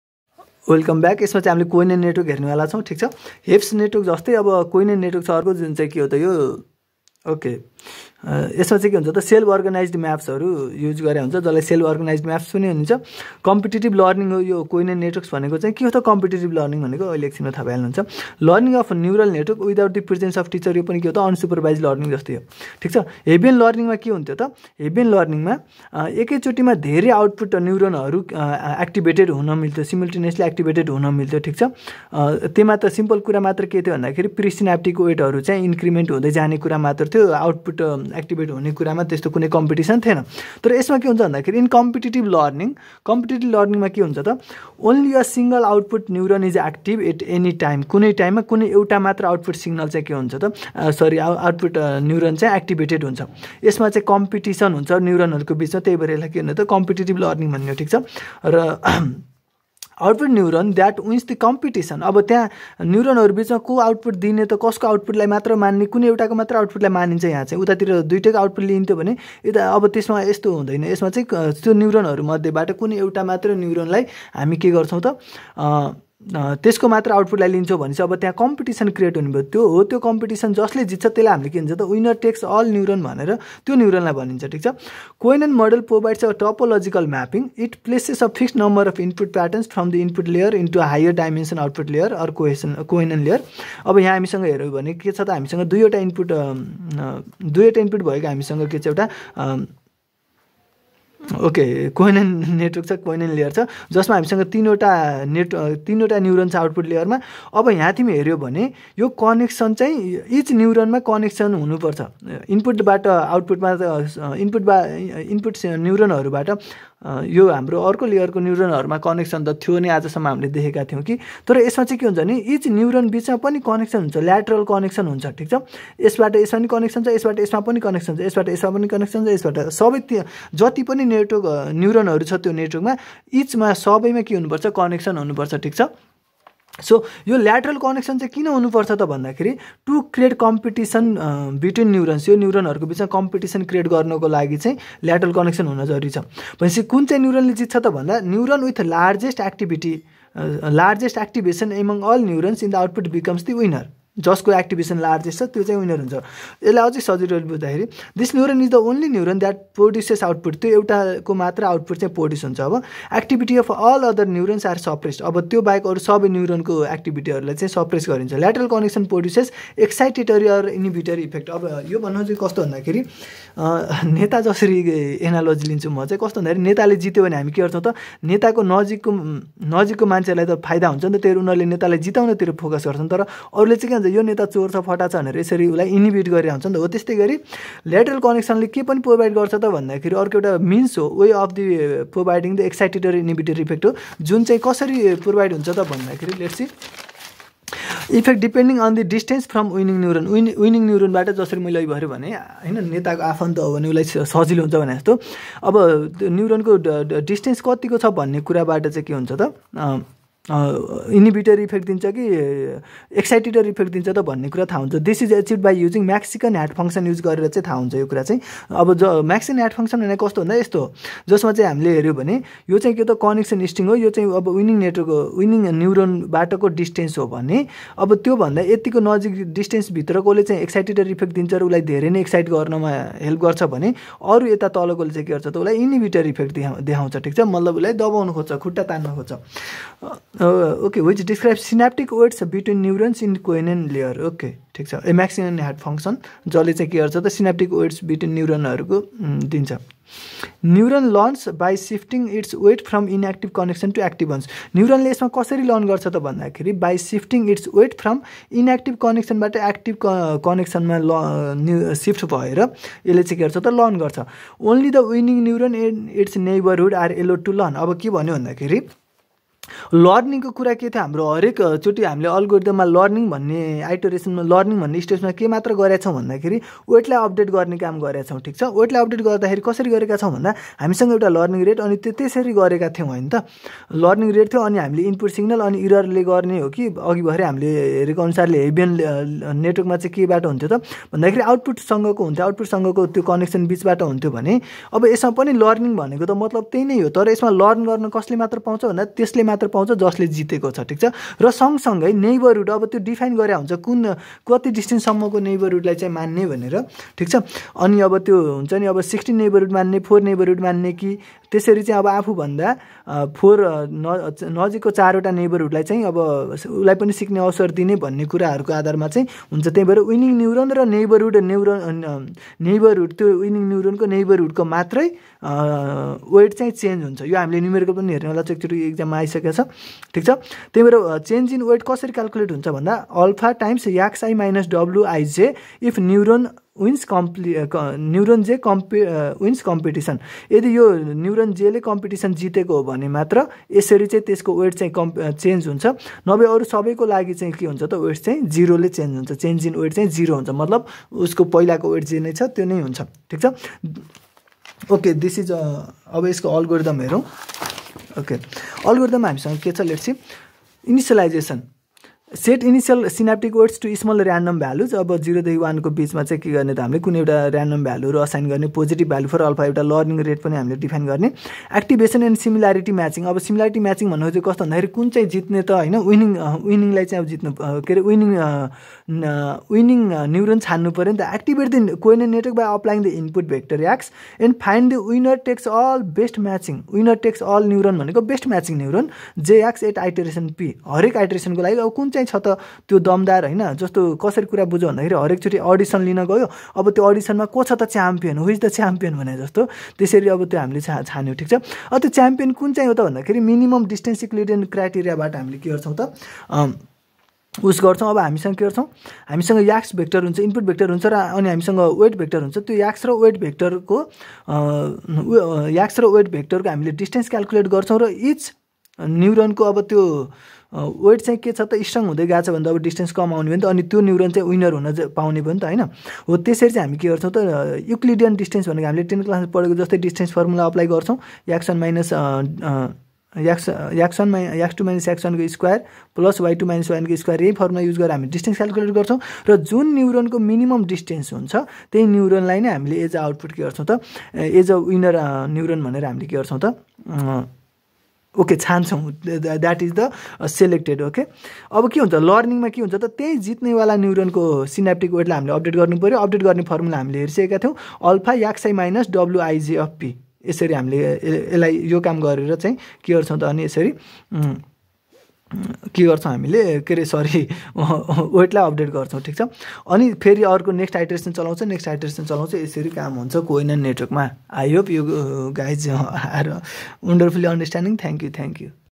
#6.7 Kohenon Network in artificial intelligence - Shiva Gyawali
Hey,
We are learning about Artificial intelligence and neural network. In this series,  we are learning about perceptron and different activation function in AI.